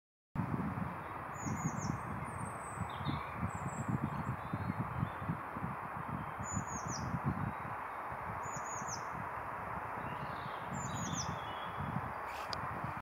Nuthatch at its nest
Nuthatch using an old tree, possible woodpecker nest, for its nest. Using mud to make hole smaller to keep out the nasties..